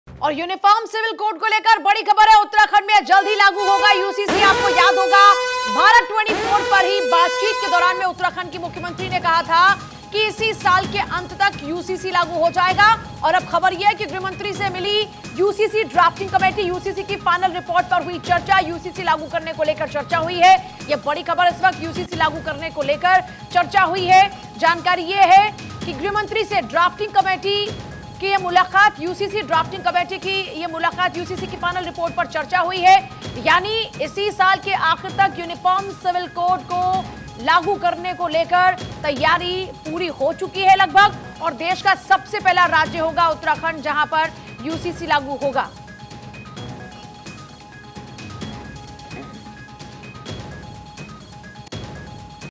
Uttarakhand में जल्द लागू होगा UCC,गृहमंत्री से मिली UCC ड्राफ्टिंग कमेटी | Amit Shah | CM Dhami
उत्तराखंड में जल्द लागू होगा UCC...गृहमंत्री से मिली UCC ड्राफ्टिंग कमेटी. UCC की फाइनल रिपोर्ट पर हुई चर्चा...UCC लागू करने को लेकर हुई चर्चा